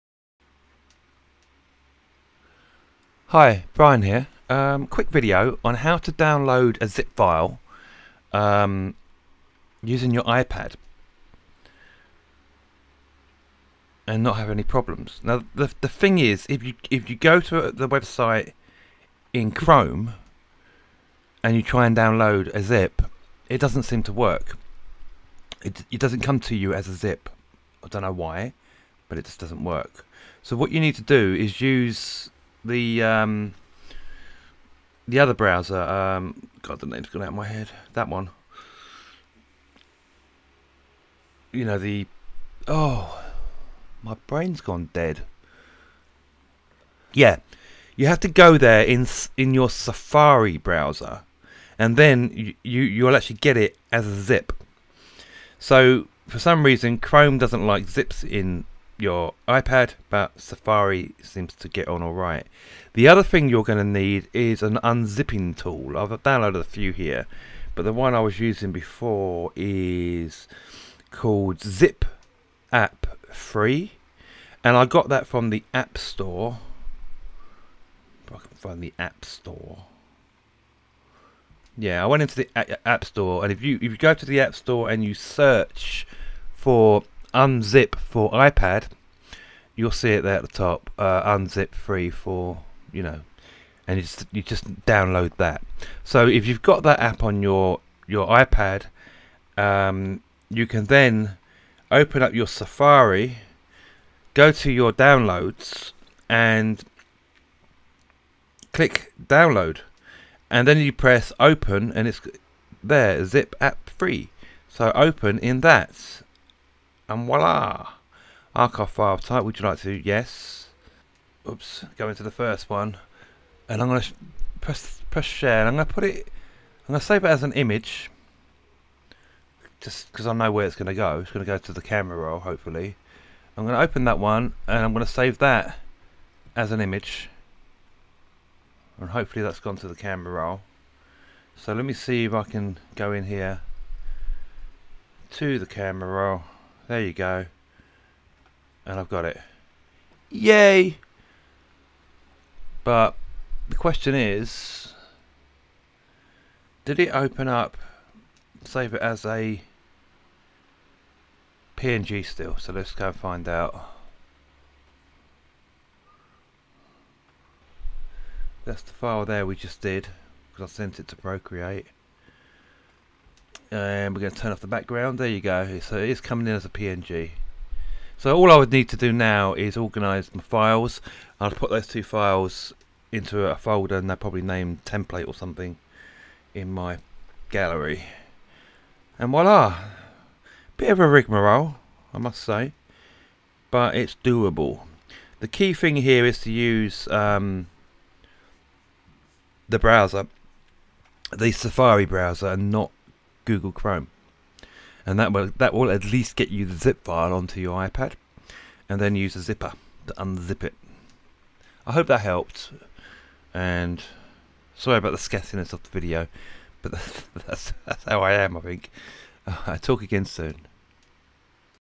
Doodle With Me - How To Download A Zip File On iPad
How To Download A Zip File On iPad it can be tricky unlike using an imac or PC so I made a video.

Stress and depression can affect so many lives in a number of different ways.

Drawing or writing can help bring the mind and energy into the now time, giving people the chance to recharge.